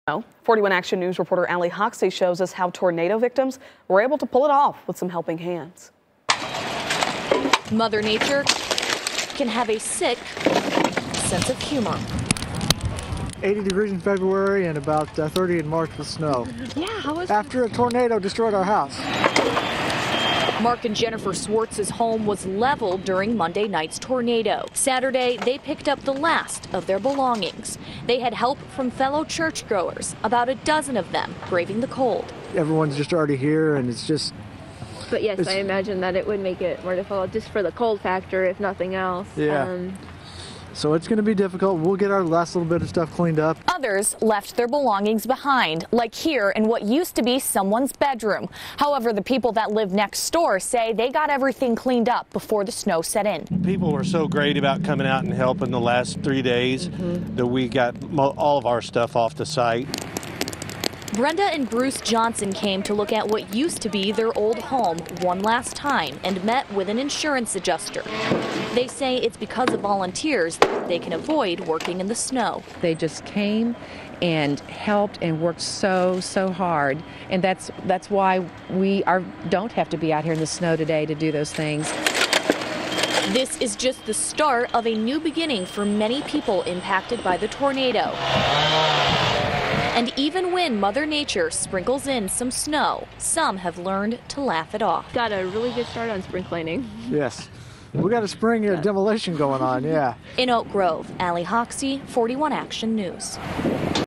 Tornado victims continue cleanup amid snow
Despite the cold temperatures, there were still people cleaning up debris and meeting with insurance adjusters in Oak Grove.